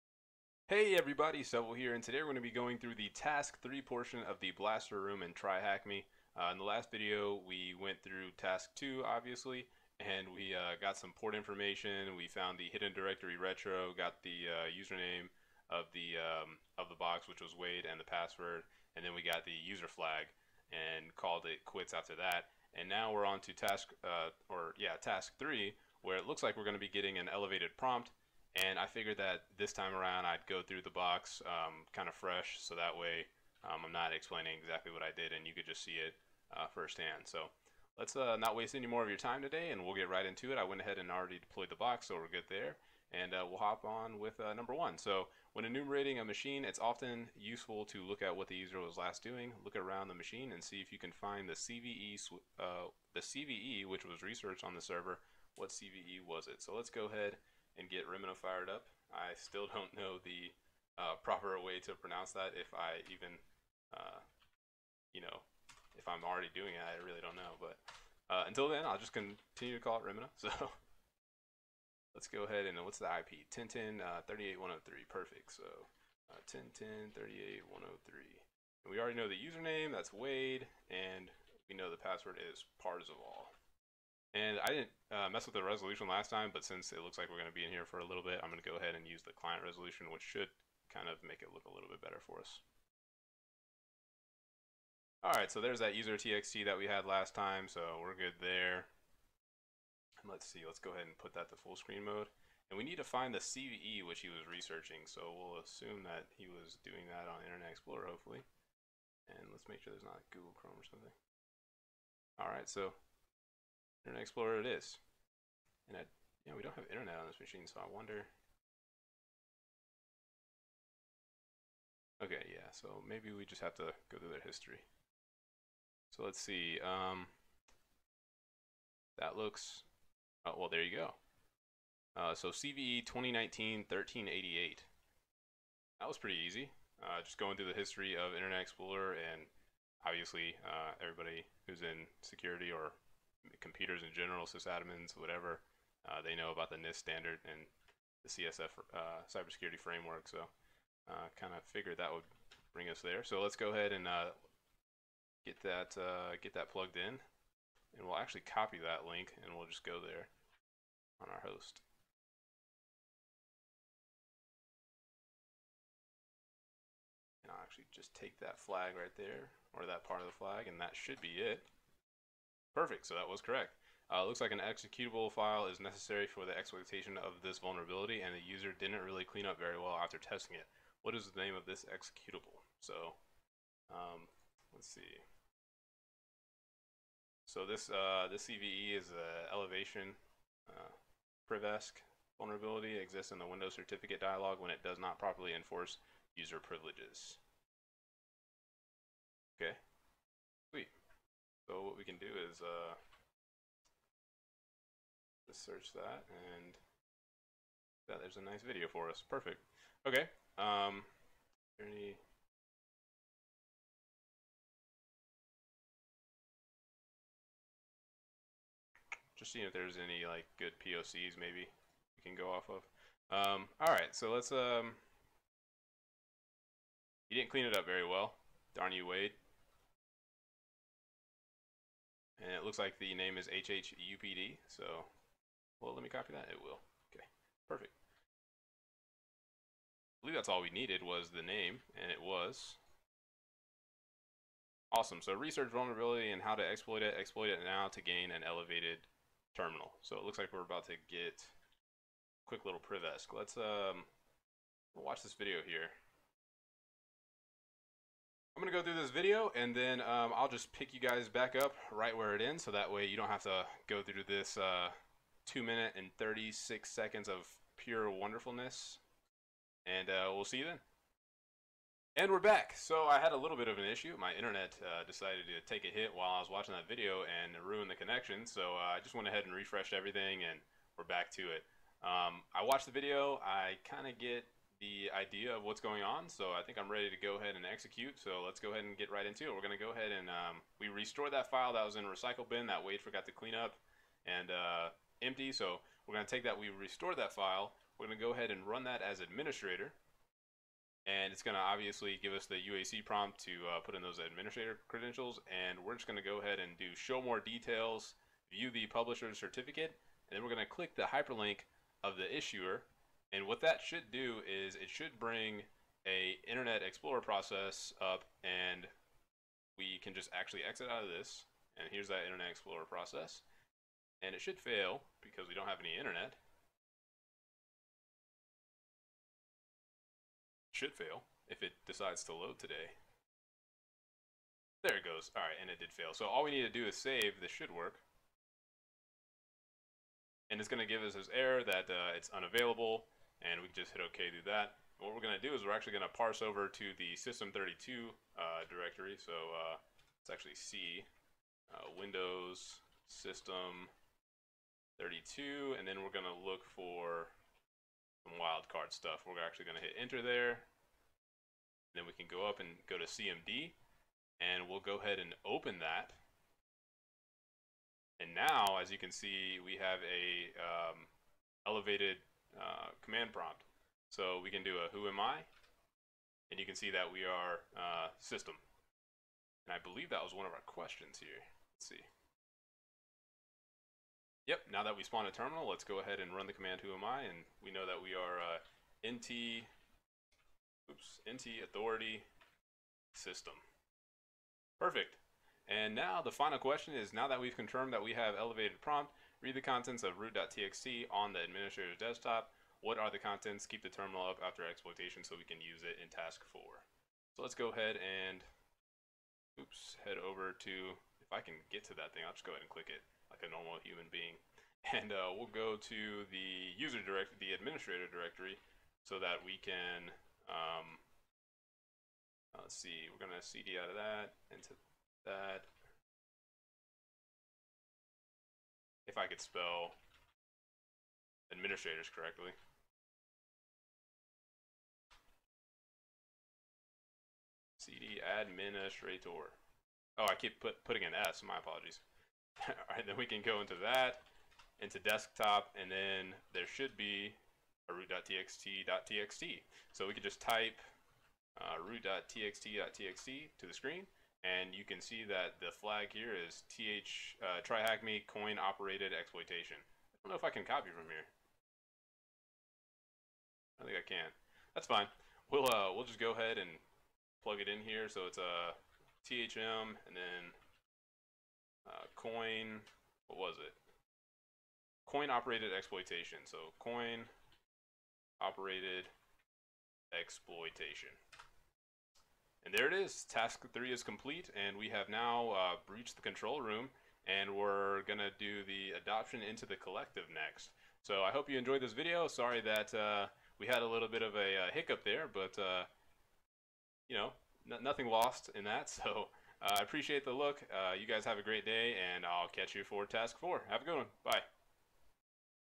TryHackMe | Blaster Task 3 | Walkthrough
A walkthrough on completing "Task 3" of the Blaster room in TryHackMe! This time we exploit the Wade's machine using a CVE he researched and and happily left for us to discover, which grants us privilege escalation.